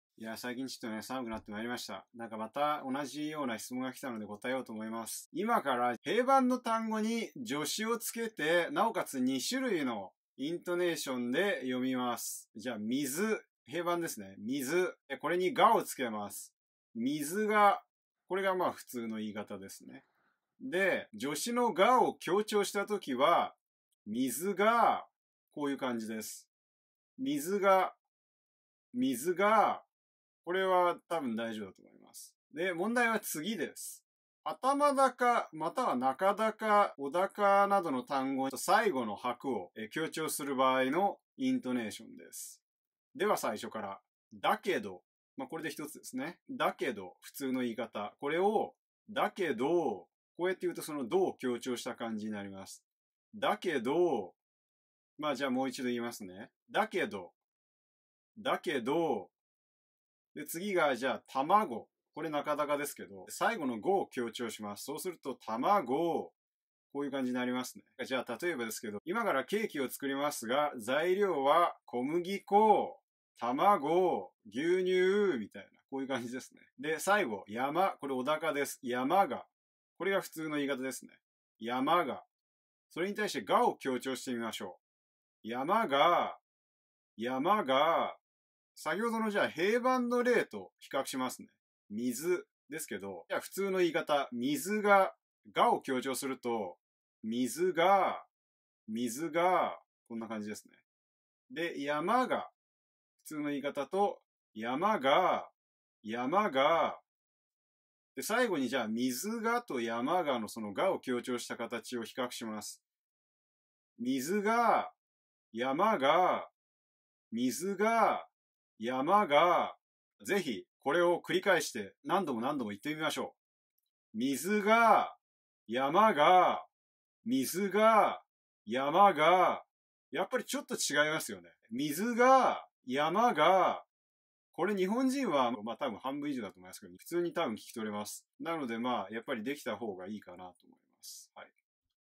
Can You Hear the Difference? 下がるイントネーションに注意！
Q.最後の拍を強調するとピッチアクセントはどうなるの？

00:03 「小高」は誤字です。「尾高」でした。

関連動画：Q.「って」がつくとアクセントが変わることはありますか？